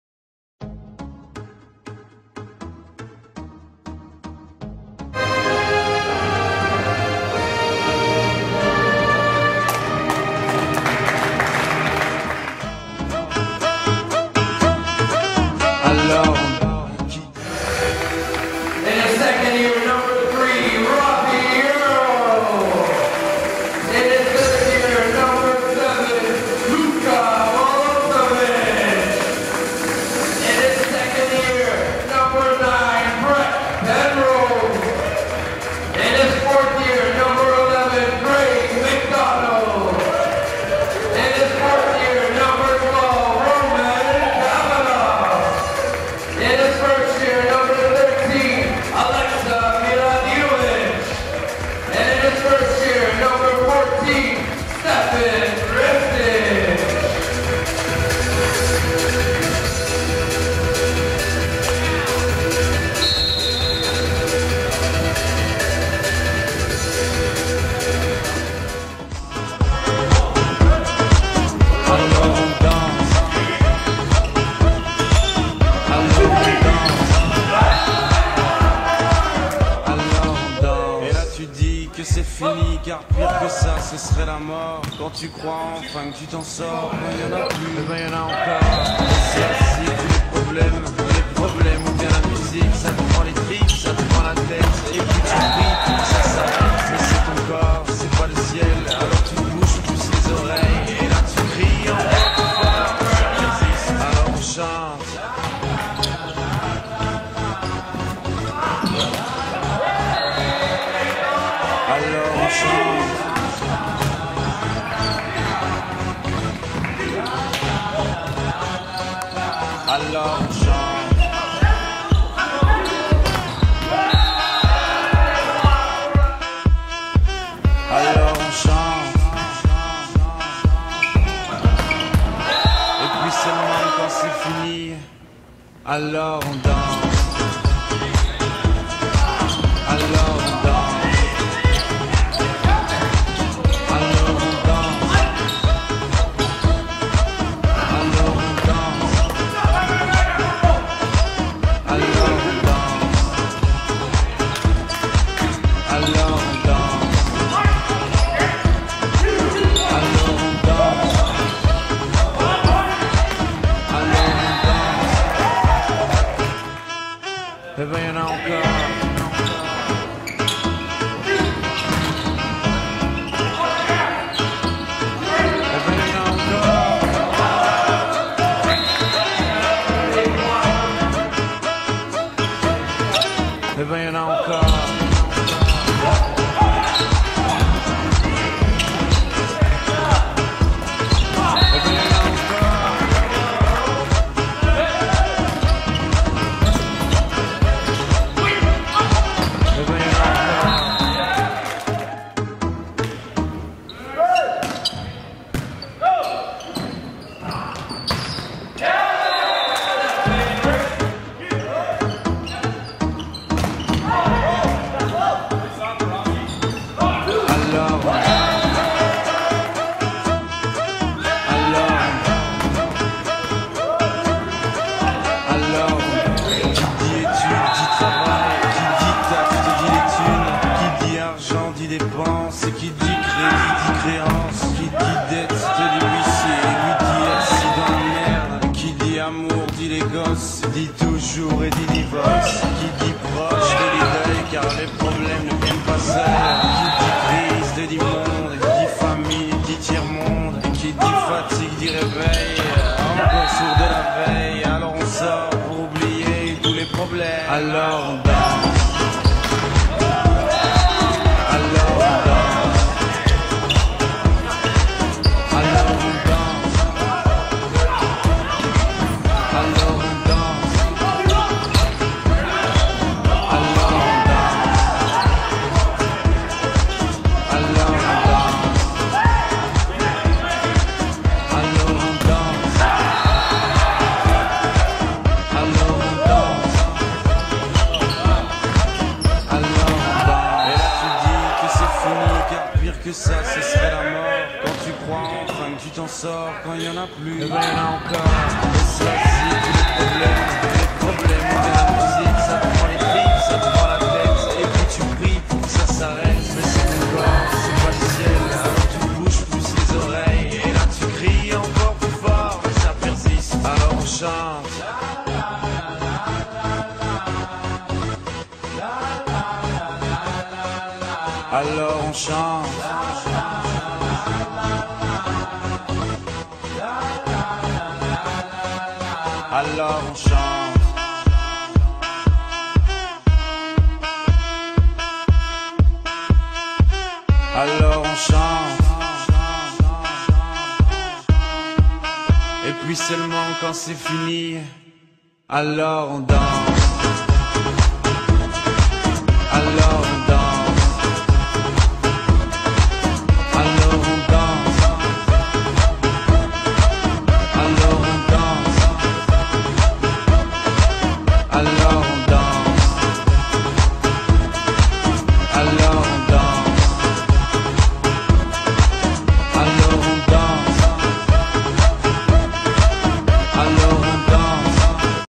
Western vs Ryerson - Final
Western and Ryerson square in final at 14 Annual Ryerson Tournament.  Western Mustangs took gold medals and Championship trophy.   They will be team to beat in OUA.